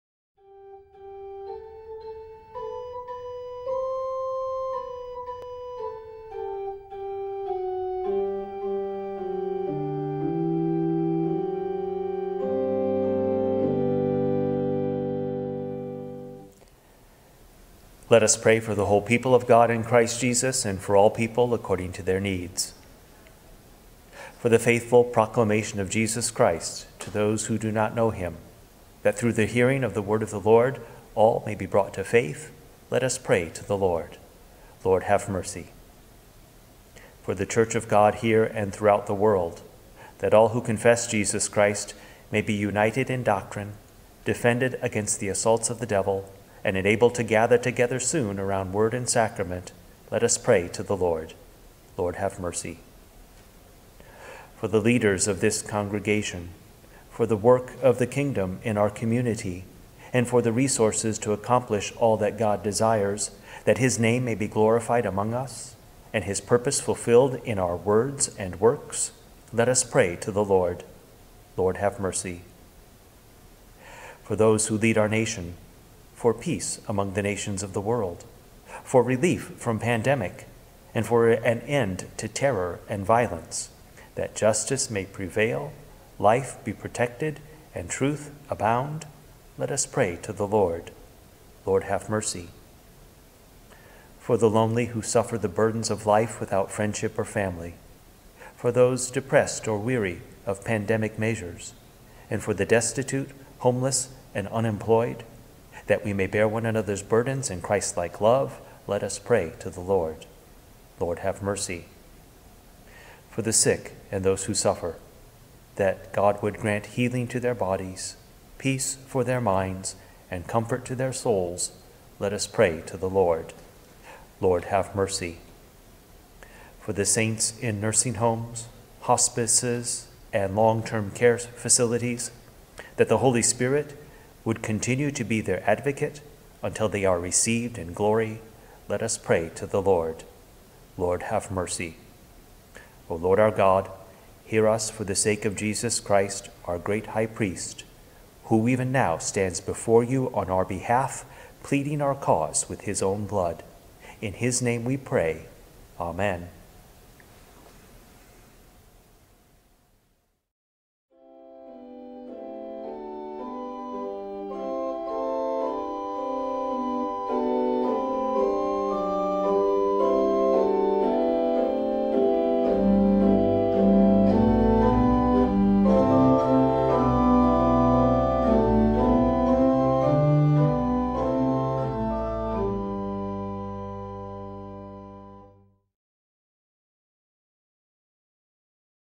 May 17, 2020 | Prayer of the Church
Pastor Anderson leads us in the Prayer of the Church for the Sixth Sunday of Easter. Lord, hear our prayer for Jesus' sake. Amen.